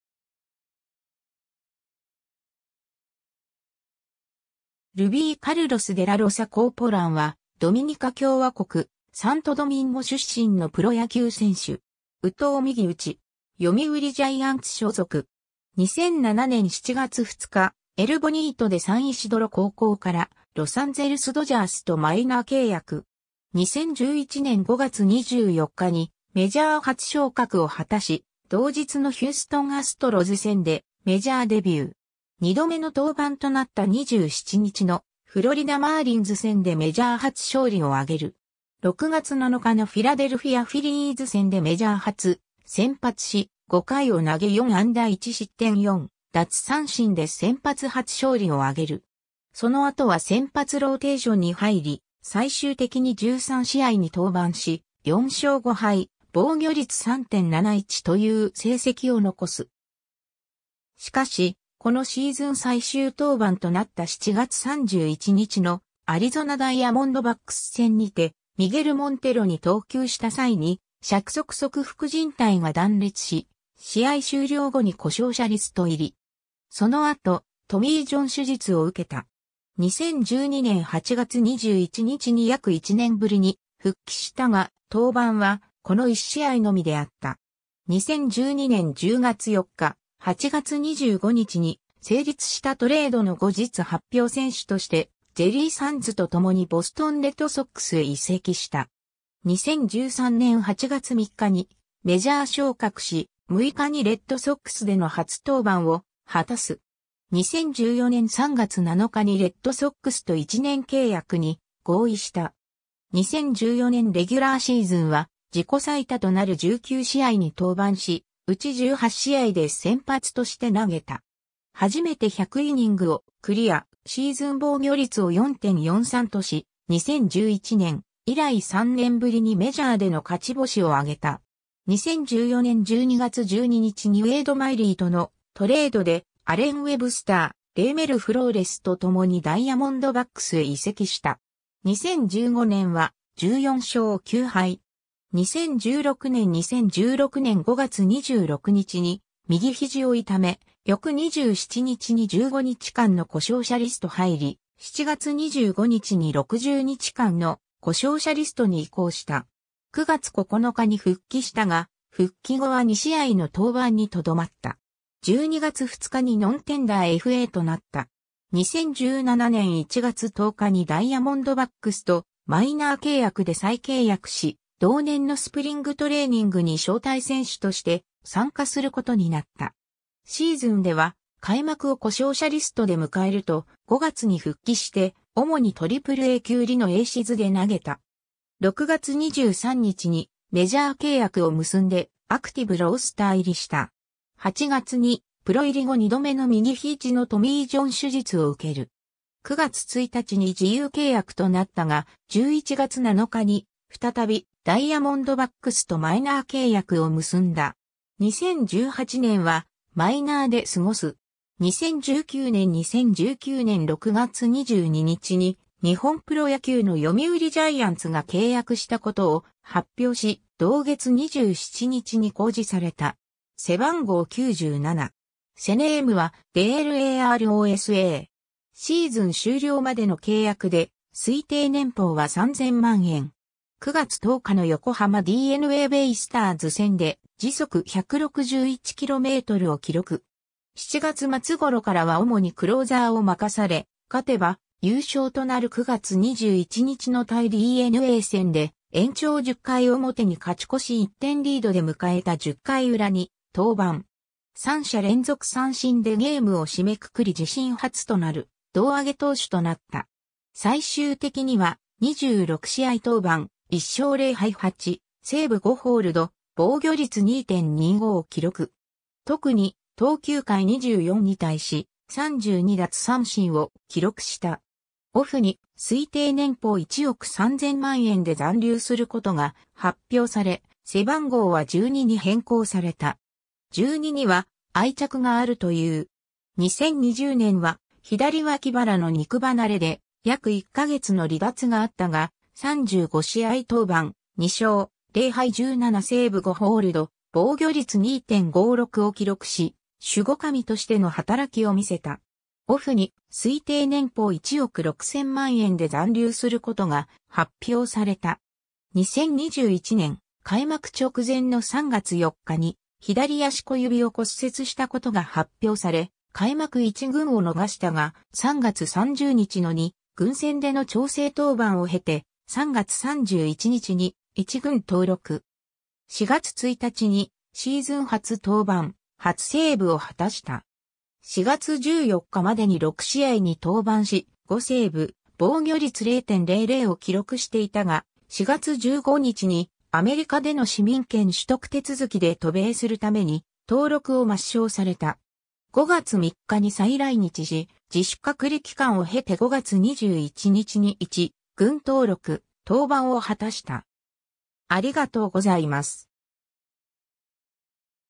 ルビー・デラロサ
ロサンゼルス・ドジャース傘下の選手
ボストン・レッドソックス傘下の選手
アリゾナ・ダイヤモンドバックス傘下の選手
ルビー・カルロス・デ・ラ・ロサ・コーポラン（Rubby Carlos De La Rosa Corporan, 1989年3月4日 - ）は、ドミニカ共和国・サントドミンゴ出身のプロ野球選手（投手）。
右投右打。
読売ジャイアンツ所属。
 2007年7月2日、エル・ボニート・デ・サンイシドロ高校からロサンゼルス・ドジャースとマイナー契約。
 2011年5月24日にメジャー初昇格を果たし、同日のヒューストン・アストロズ戦でメジャーデビュー。
2度目の登板となった27日のフロリダ・マーリンズ戦でメジャー初勝利を挙げる。
6月7日のフィラデルフィア・フィリーズ戦でメジャー初先発し、5回を投げ4安打1失点4奪三振で先発初勝利を挙げる。
その後は先発ローテーションに入り、最終的に13試合に登板（先発は10試合）し、4勝5敗・防御率3.71という成績を残す。
しかし、このシーズン最終登板となった7月31日のアリゾナ・ダイヤモンドバックス戦にて、ミゲル・モンテロに投球した際に尺側側副靱帯が断裂し、試合終了後に故障者リスト入り。
その後トミー・ジョン手術を受けた。
 2012年8月21日に約1年ぶりに復帰したが、登板はこの1試合のみであった。
 2012年10月4日、8月25日に成立したトレードの後日発表選手として、ジェリー・サンズと共にボストン・レッドソックスへ移籍した。
 2013年8月3日にメジャー昇格し、6日にレッドソックスでの初登板を果たす。
 2014年3月7日にレッドソックスと1年契約に合意した。
2014年レギュラーシーズンは、自己最多となる19試合に登板し、うち18試合で先発として投げた。
初めて100イニングをクリア、シーズン防御率を4.43とし、2011年以来3年ぶりにメジャーでの勝ち星を挙げた。
 2014年12月12日にウェイド・マイリーとのトレードで、アレン・ウェブスター、レイメル・フローレスと共にダイヤモンドバックスへ移籍した。
 2015年は、14勝9敗。
 2016年 2016年5月26日に右肘を痛め、翌27日に15日間の故障者リスト入り、7月25日に60日間の故障者リストに移行した。
9月9日に復帰したが、復帰後は2試合の登板にとどまった。
12月2日にノンテンダーFAとなった。
 2017年1月10日にダイヤモンドバックスとマイナー契約で再契約し、同年のスプリングトレーニングに招待選手として参加することになった。
シーズンでは開幕を故障者リストで迎えると、5月に復帰して主にAAA級リノ・エーシズで投げた。
6月23日にメジャー契約を結んでアクティブ・ロースター入りした。
8月にプロ入り後2度目の右肘のトミー・ジョン手術を受ける。
9月1日に自由契約となったが、11月7日に再びダイヤモンドバックスとマイナー契約を結んだ。
 2018年は、マイナーで過ごす。
 2019年 2019年6月22日に日本プロ野球の読売ジャイアンツが契約したことを発表し、同月27日に公示された。
背番号97。
背ネームはDE LA ROSA。
シーズン終了までの契約で、推定年俸は3,000万円。
9月10日の横浜DeNAベイスターズ戦（横浜スタジアム）で161km/hを記録。
7月末頃からは主にクローザーを任され、勝てば優勝となる9月21日の対DeNA戦（横浜スタジアム）で延長10回表に勝ち越し1点リードで迎えた10回裏に登板。
三者連続三振でゲームを締めくくり自身初となる胴上げ投手となった。
最終的には、26試合登板、1勝0敗8セーブ5ホールド、防御率2.25を記録。
特に、投球回24に対し、32奪三振を記録した。
オフに、推定年俸1億3,000万円で残留することが発表され、背番号は12に変更された。
「12」には愛着があるという。
 2020年は左脇腹の肉離れで約1ヶ月の離脱があったが、35試合登板、2勝0敗17セーブ5ホールド、防御率2.56を記録し、守護神としての働きを見せた。
オフに、推定年俸1億6,000万円で残留することが発表された。
 2021年、開幕直前の3月4日に左足小指を骨折したことが...